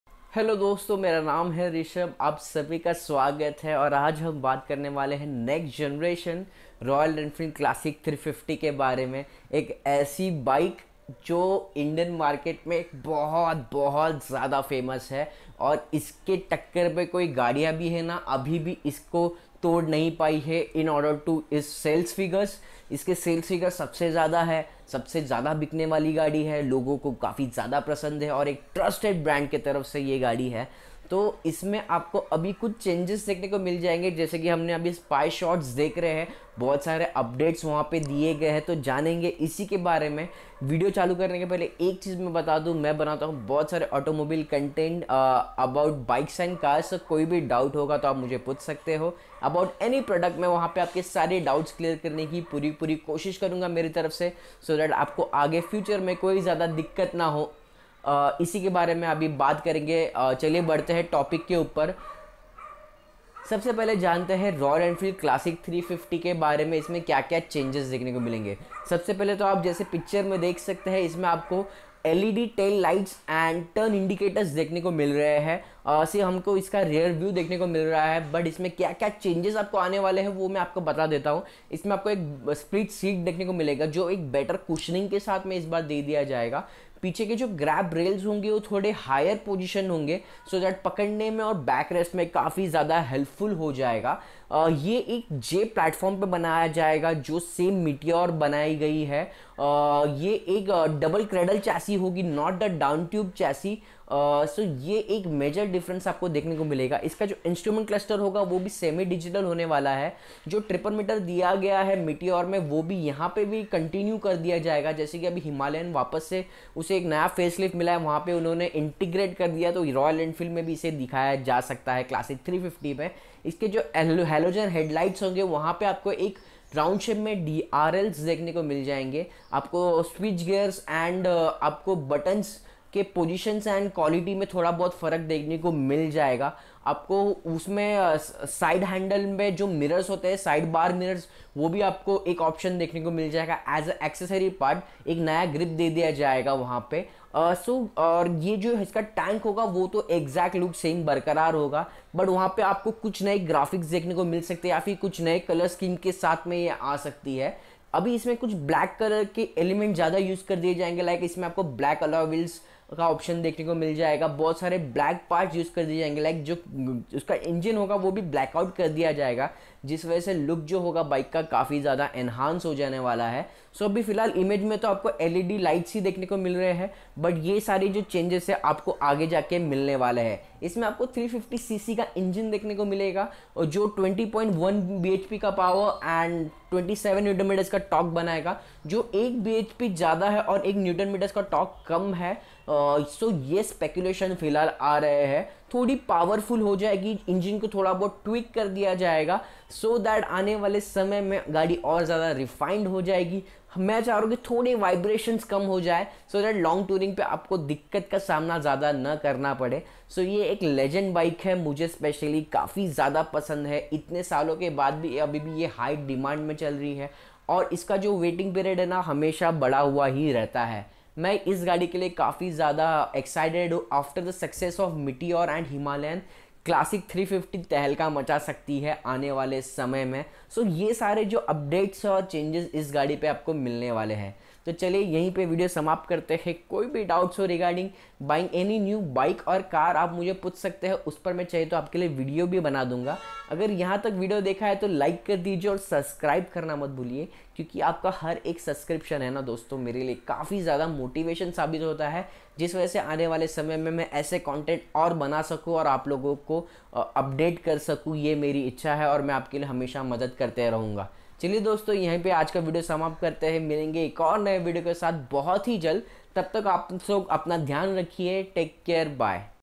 2021 Next Gen Royal Enfield Classic 350 Spied - Launching in INDIA Soon ! The Garage Official
Recently, two test mules of the next-gen Classic 350 were spotted testing again, and this time in their production-spec avatar.

Thumbnail picture credits - MotorBeam